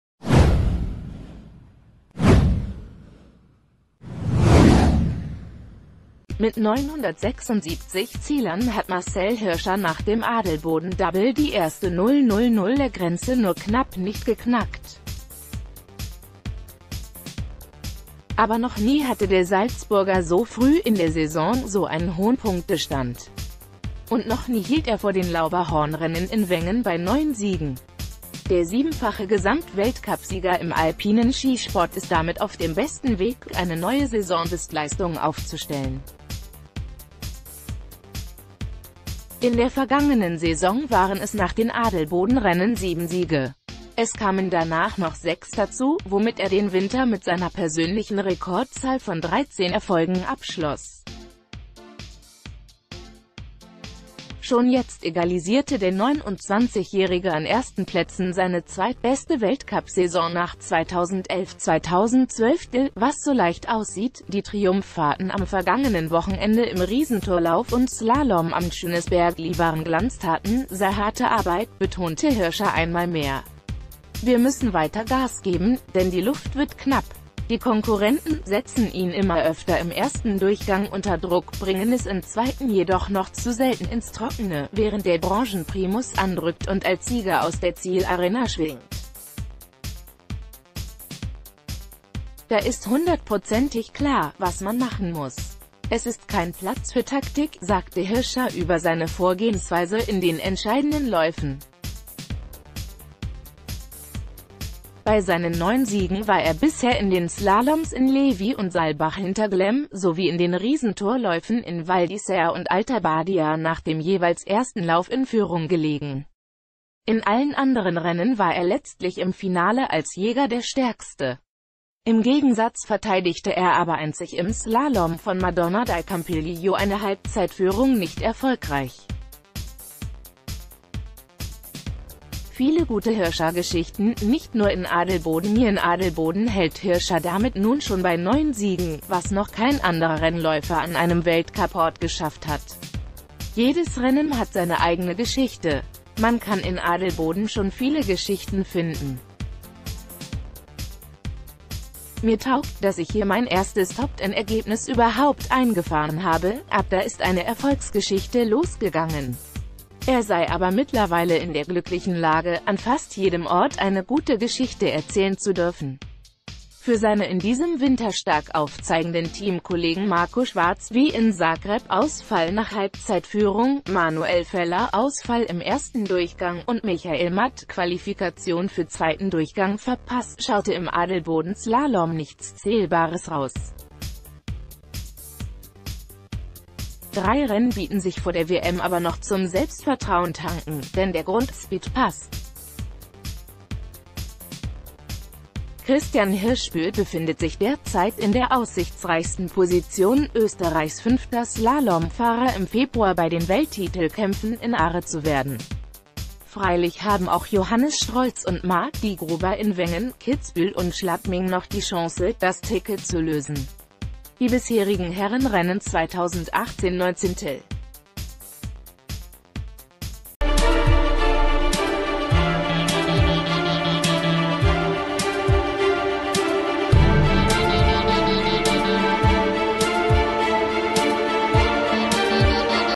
Bester Hirscher aller Zeiten: Noch nie so früh so viele Punkte und Siege
Mit 976 Zählern hat Marcel Hirscher nach dem Adelboden-Double die 1.000er-Grenze nur knapp nicht geknackt. Aber noch nie hatte der Salzburger so früh in der Saison so einen hohen Punktestand. Und noch nie hielt er vor den Lauberhorn-Rennen in Wengen bei neun Siegen. Der siebenfache Gesamtweltcupsieger im alpinen Skisport ist damit auf dem besten Weg, eine neue Saisonbestleistung aufzustellen. In der vergangenen Saison waren es nach den Adelboden-Rennen sieben Siege. Es kamen danach noch sechs da...